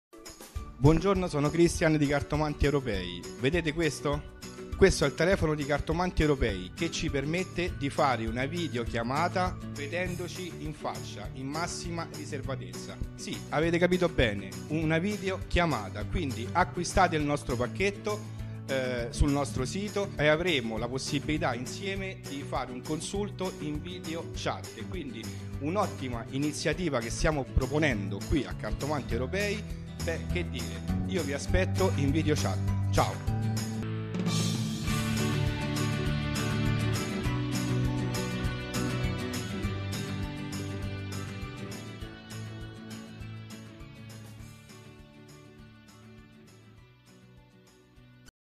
Video-CONSULTI privati con i Cartomanti Europei - Cristian
✅Hai mai desiderato poter guardare negli occhi il cartomante durante il consulto?! Oppure vedere le carte della stesura?!✅
👉Dai Cartomanti Europei ora è possibile effettuare un Video-CONSULTO privato con il tuo cartomante di fiducia!!

❓COME FARE❓
Basta avere Whatsapp o Facebook installato sul proprio cellulare e il gioco è fatto!